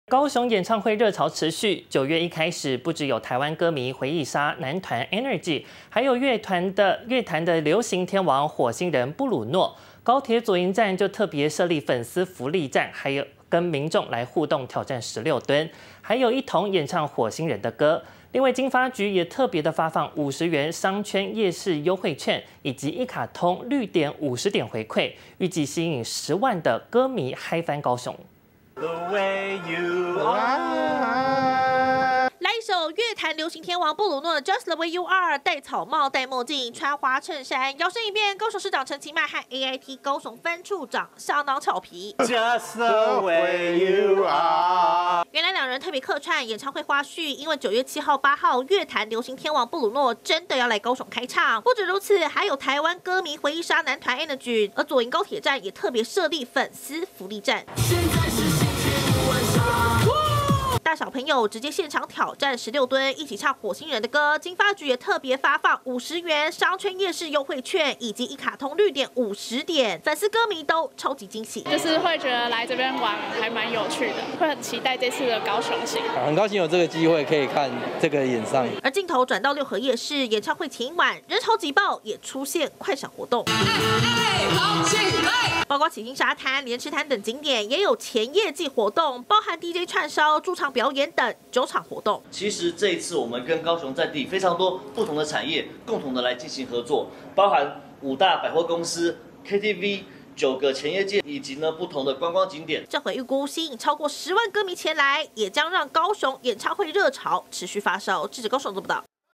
布魯諾.Energy登高雄開唱 高鐵站設「福利站」
高雄演唱會熱潮持續，九月一開始，不只有台灣歌迷回憶殺男團 Energy，還有樂壇流行天王火星人布魯諾，高鐵左營站特別設立「粉絲福利站」和民眾互動「挑戰16蹲」，還有一同演唱火星人的歌，另外經發局也特別發放50元商圈夜市優惠券，以及一卡通綠點50點回饋，預計吸引10萬歌迷嗨翻高雄。